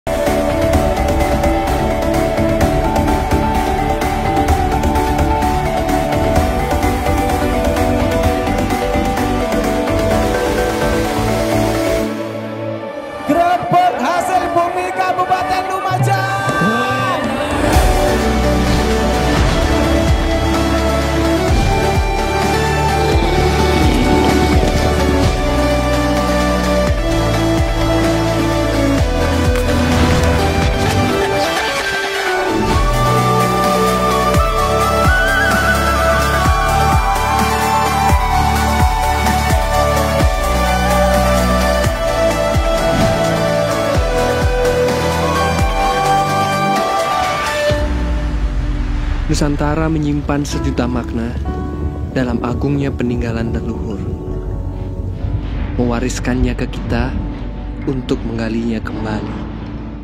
Prosesi Harjalu 767
Prosesi Hari Jadi Lumajang Yang Ke 767 yang di selenggarakan di Alun-alun Kota Lumajang.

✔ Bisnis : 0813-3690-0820

harjalu, harjalu 2022, harjalu lumajang, harjalu lumajang 2022, harjalu 767, kuda kencak harjalu